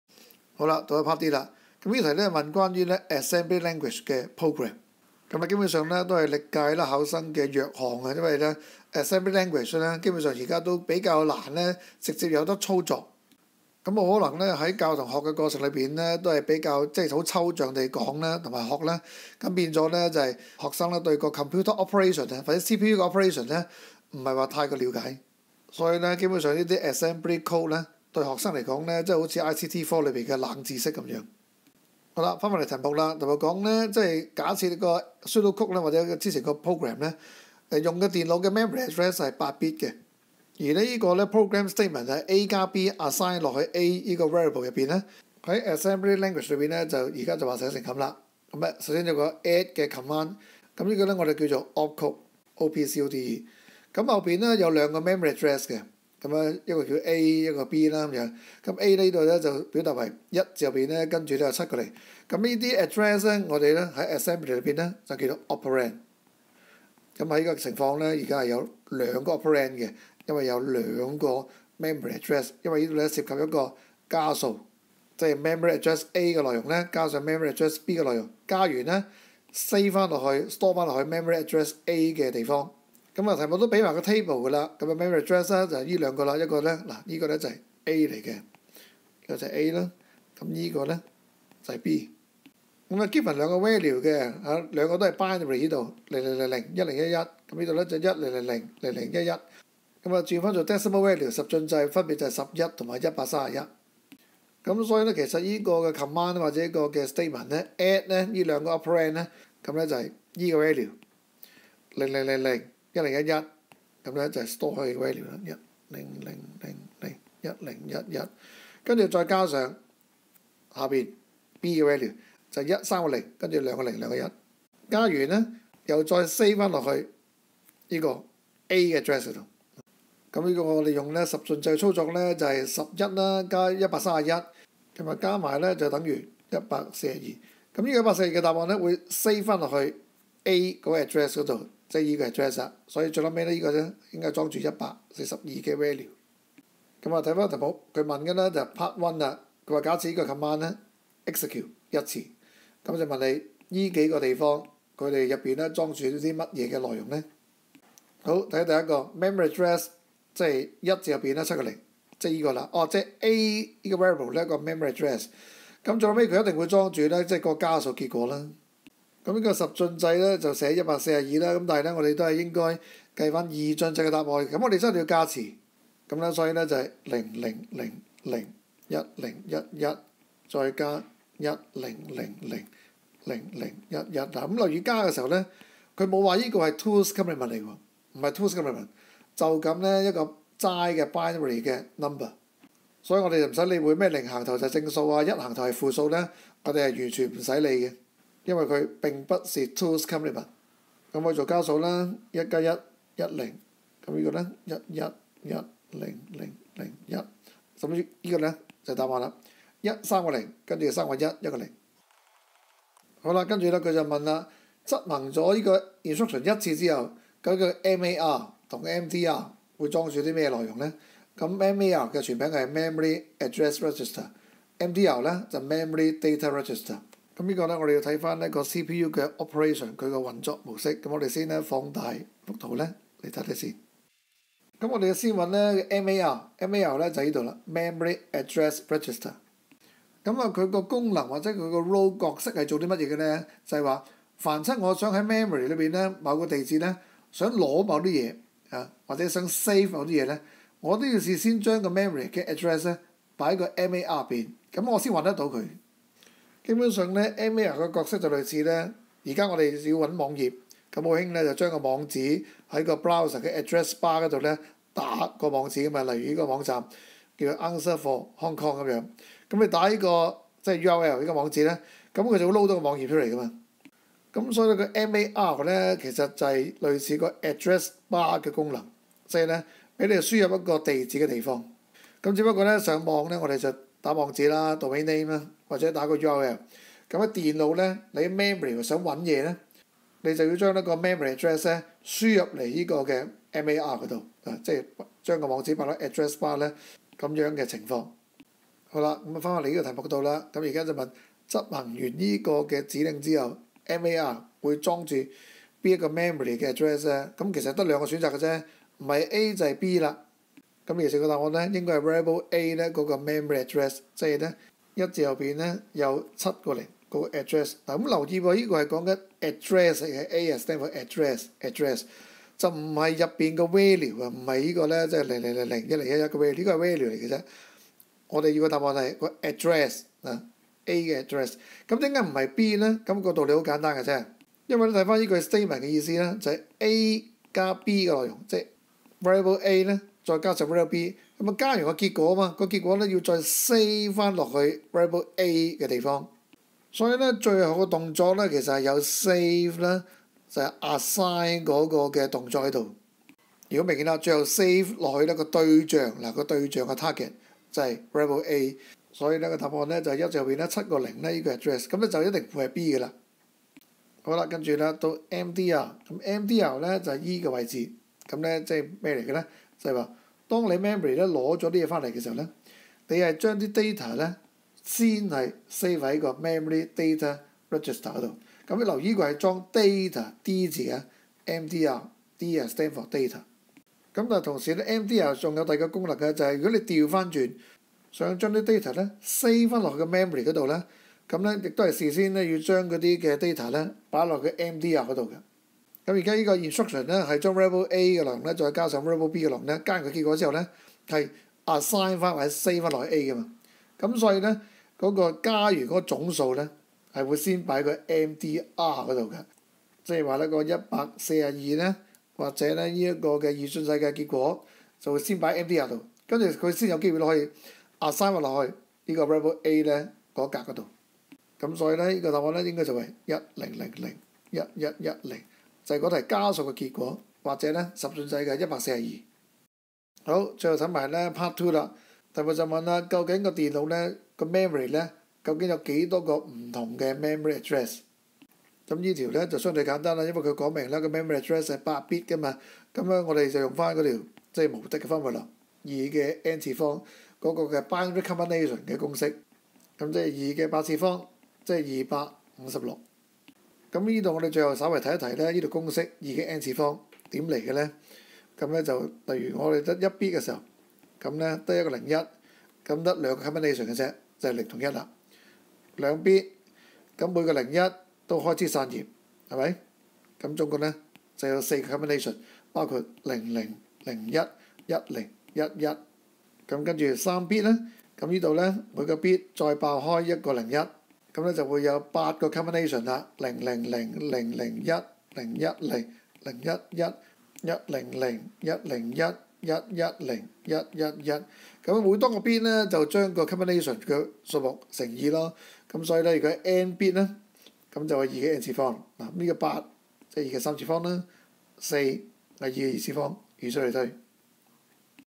DSE 2012 ICT Paper 1B Q3d - Assembly instruction (memory addresses, MAR, MDR)
MAR = Memory Address Register
MDR = Memory Data Register

在各選修科中，ICT 應試的補充教材、練習，實在少，甚至乎是冇。所以，我嘗試作一點平衡，如你期望睇到某年某題 LQ 的解釋，歡迎把年份及題號留在 comment 中👌🏻

更多 DSE ICT 應試支援，歡迎 click 以下網站 😍